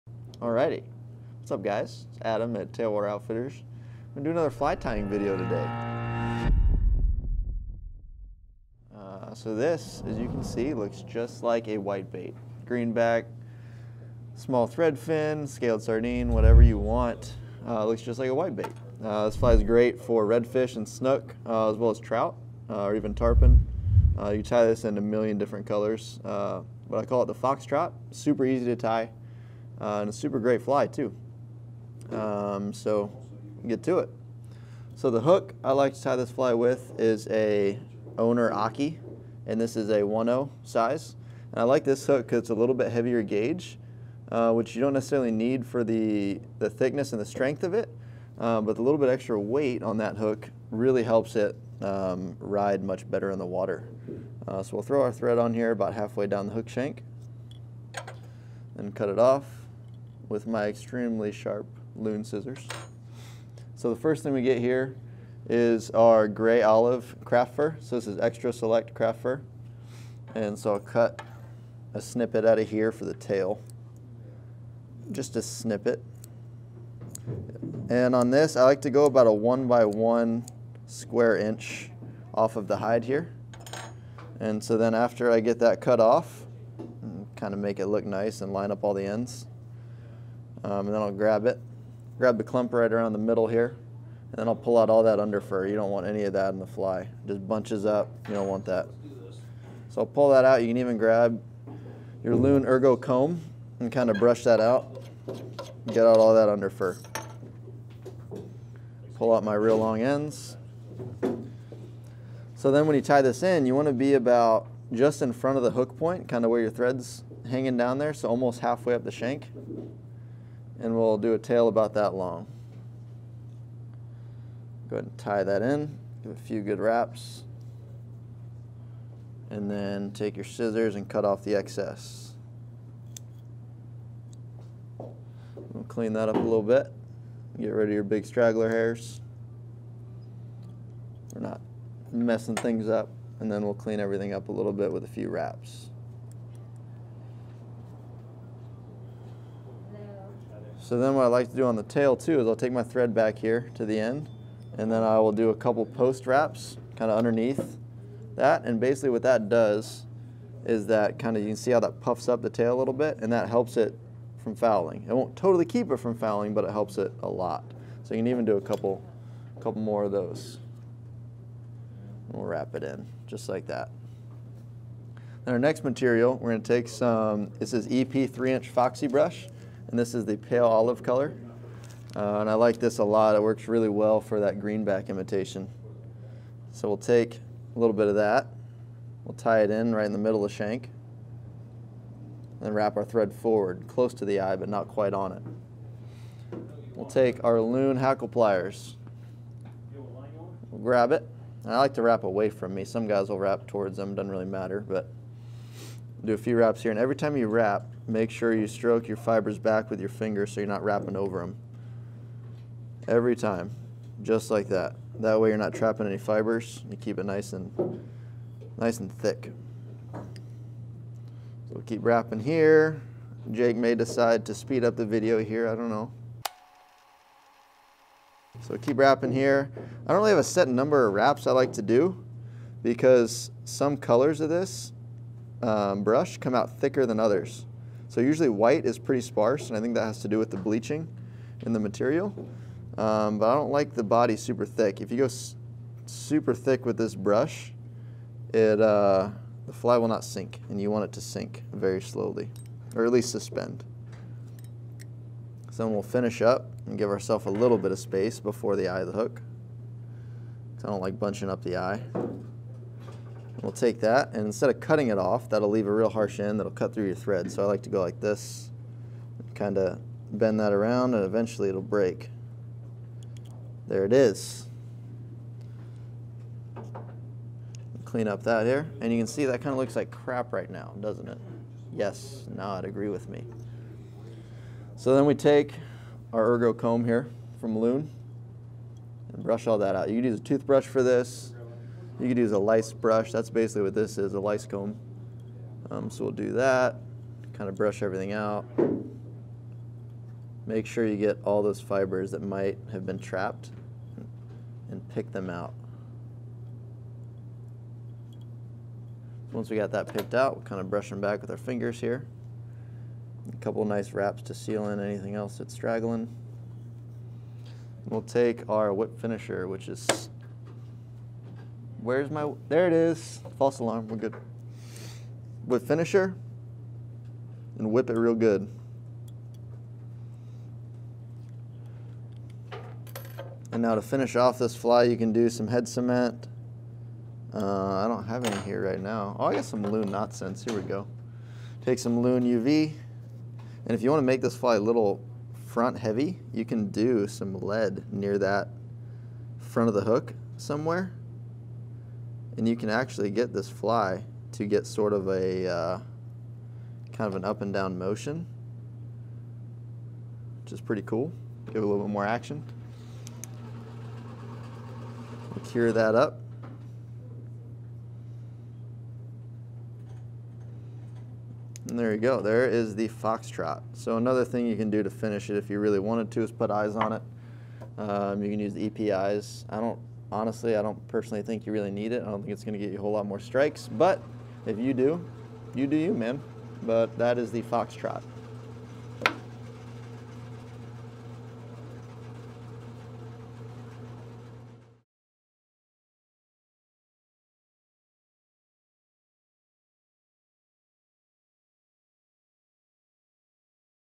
How to Tie the FoxTrot | Best All Around Inshore Fly?
Learn how to tie the FoxTrot with Adam! This is an amazing fly for many fish. You can tie it in so many sizes and colors to imitate glass minnows, scaled sardines, mullet, ladyfish, etc. It works great for redfish, snook, and trout, but will also trick tarpon and cobia, and even offshore fish like mahi, tripletail, and bonito. I most commonly tie this fly on a 1/0 Owner Aki. Being a heavier hook, it makes the fly track very well. It's super simple to tie and something that you can quickly fill a box with!

For an in-person how to for this fly or any other, come by and see us!

Shop in store at
30801 US Highway 19 N